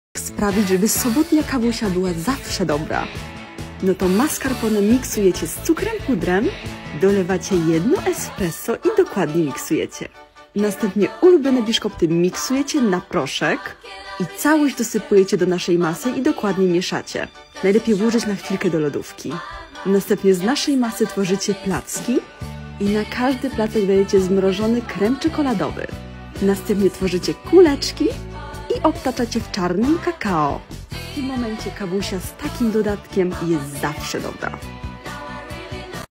Bez takiego deseru żadna kawusia nie będzie smakować 🤩
Z takim deserem nie tylko w sobotę kawusia będzie dobra 😀
250g serka mascarpone,
200g podłużnych biszkoptów,
1 espresso ( 20ml )
60g cukru pudru
Kakao do obtaczania
Krem czekoladowy ( twardy z lodówki )